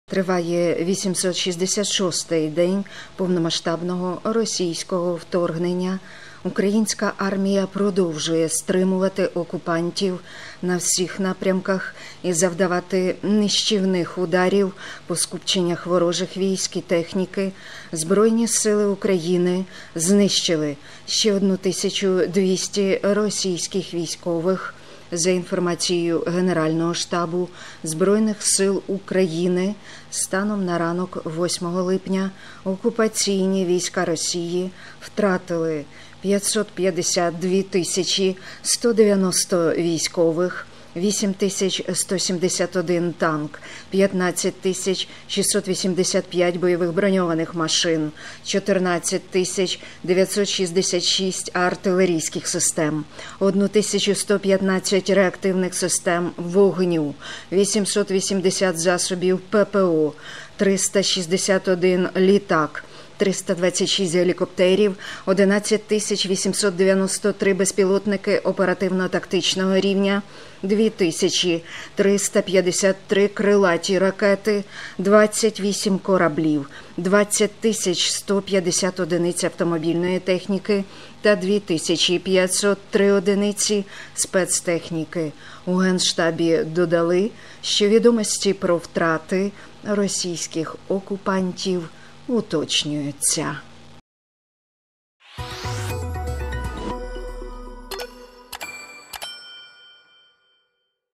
☠️💣Генштаб ЗСУ: втрати Росії в Україні станом на 8 липня
Триває 866 день повномасштабного російського вторгнення.
Українська армія продовжує стримувати окупантів на всіх напрямках і завдавати нищівних ударів по скупченнях ворожих військ та техніки. ЗСУ знищили ще 1 200 російських військових.
За інформацією Генерального штабу Збройних сил України, станом на ранок 8 липня окупаційні війська Росії втратили 552 190 військових, 8 171 танк,
15 685 бойових броньованих машин,  14 966 артилерійських систем, 1 115 реактивних систем залпового вогню, 880 засобів ППО, 361 літак, 326 гелікоптерів, 11 893 безпілотники оперативно- тактичного рівня, 2 353 крилаті
ракети, 28 кораблів, 20 150 одиниць автомобільної техніки та 2 503 одиниці спецтехніки.
У Генштабі додали, що відомості про втрати російських окупантів уточнюються.